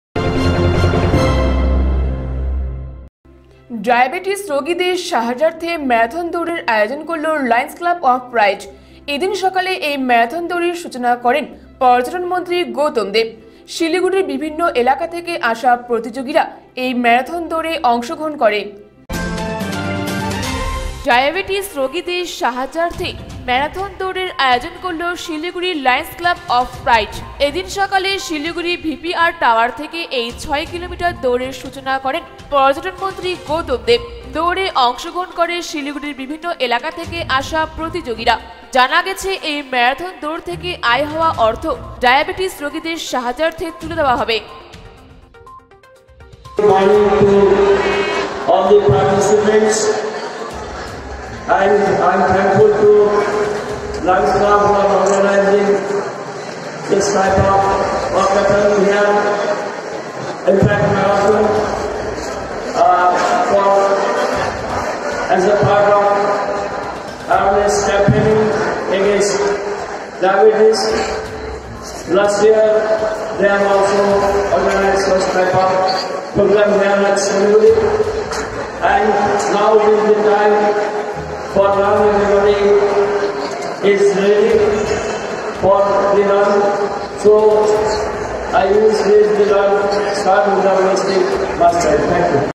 ডায়াবেটিস রোগীদের সাহায্যার্থে ম্যারাথন দৌড়ের আয়োজন ৬ কিমি দৌড়ের সূচনা করেন পর্যটন মন্ত্রী গৌতম দ
ডায়াবেটিস রোগীদের সাহায্যার্থে ম্যারাথন দৌড়ের আয়োজন ৬ কিমি দৌড়ের সূচনা করেন পর্যটন মন্ত্রী গৌতম দেব দৌড়ে অংশগ্রহণ করে শিলিগুড়ির বিভিন্ন এলাকা থেকে আসা প্রতিযোগিরা এই দৌড় থেকে আয় হওয়া অর্থ ডায়াবেটিস রোগীদের সাহায্যার্থে তুলে দেওয়া হবে